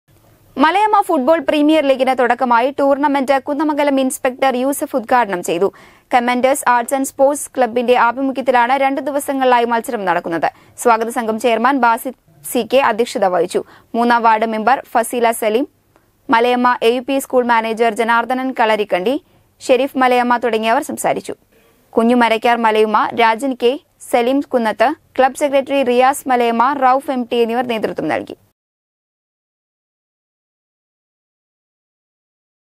മലയമ്മ ഫുട്ബോൾ പ്രീമിയർ ലീഗിന് തുടക്കമായി.
മലയമ്മ ഫുട്ബോൾ പ്രീമിയർ ലീഗിന് തുടക്കമായി. ടൂർണ്ണമെൻ്റ്  കുന്നമംഗലം ഇൻസ്പെക്ടർ യൂസഫ്  ഉദ്ഘാടനം ചെയ്തു  ഞങ്ങളുടെ ചാനൽ സോഷ്യൽ മീഡിയ ചാനലുകൾ  ലഭിക്കുവാൻ